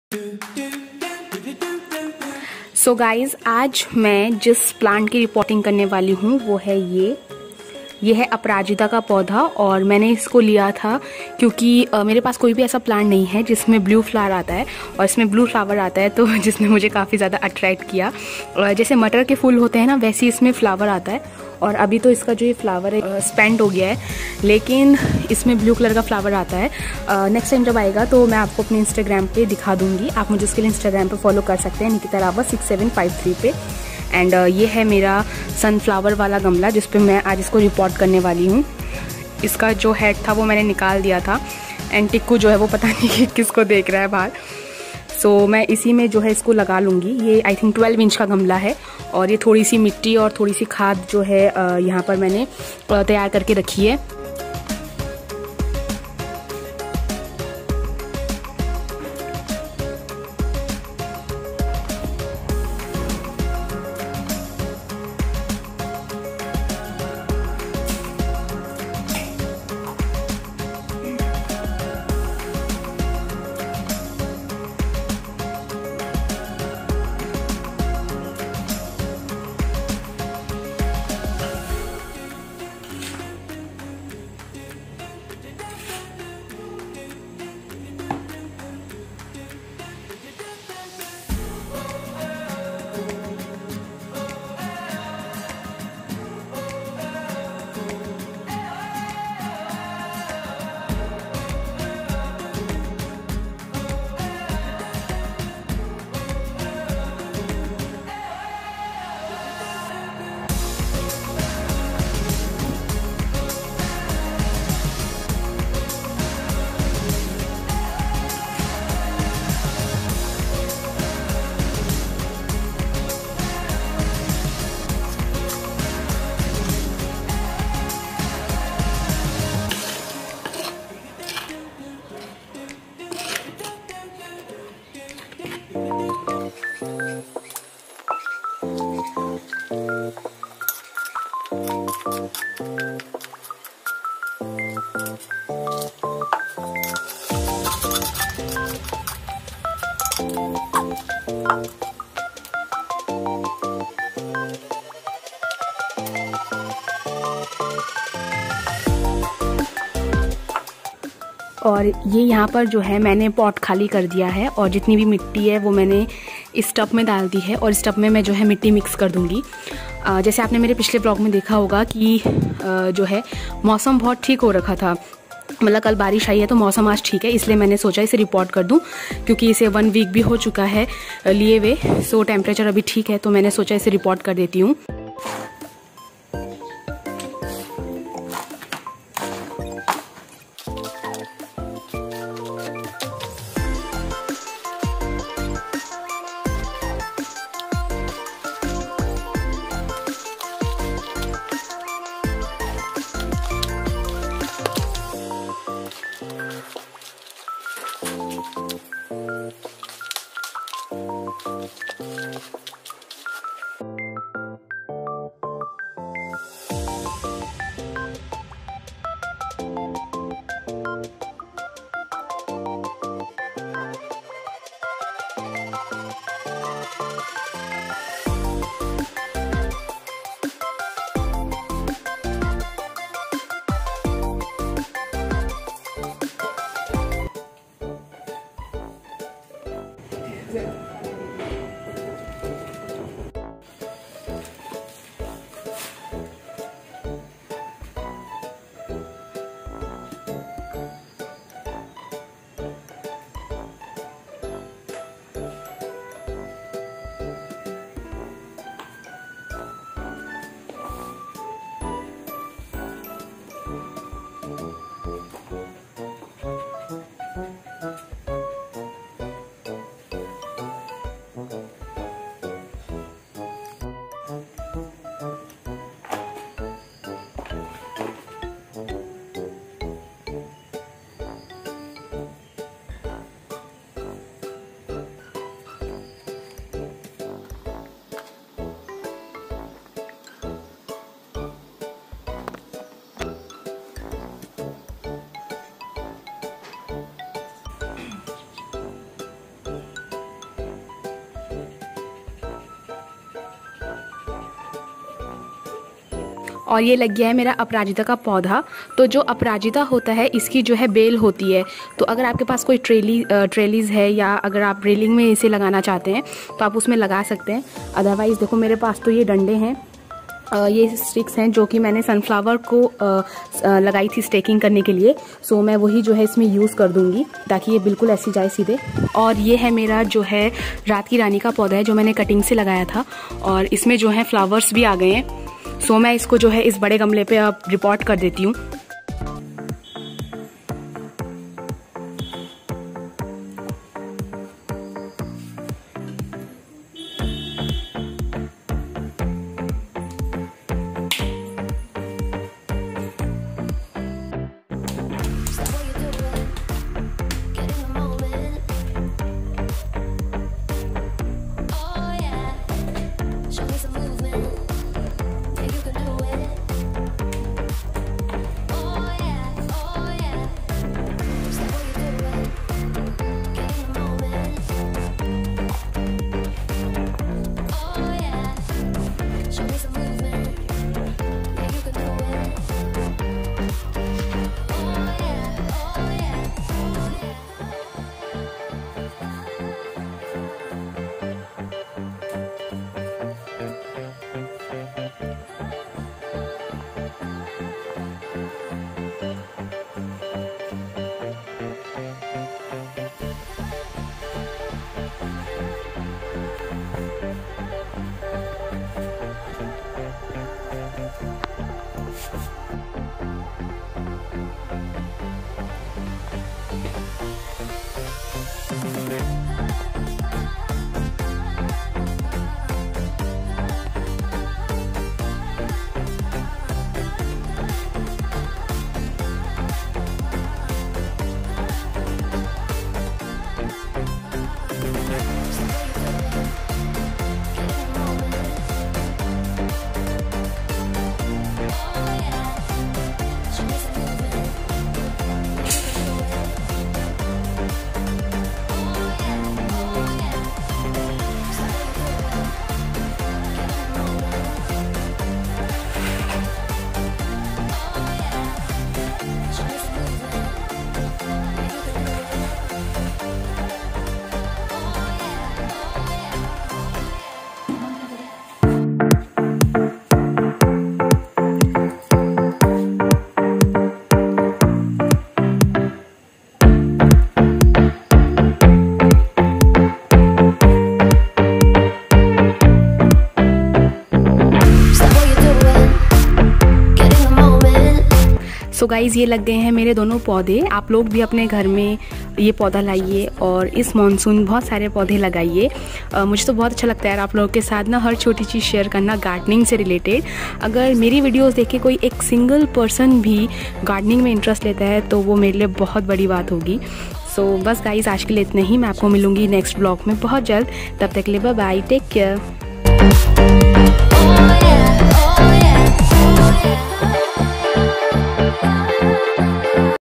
[eng sub] Repotting of Aparajita(Clitoria terntea) and Raat ki Rani (Night Jasmine) || monsoon plant
Repotting of aparajita (Clitoria terntea) , raatkirani (Night jasmine) plants with mypet 🐶

I love to share all the small things related to my gardening , nature , plants etc. with you all😍😍😍.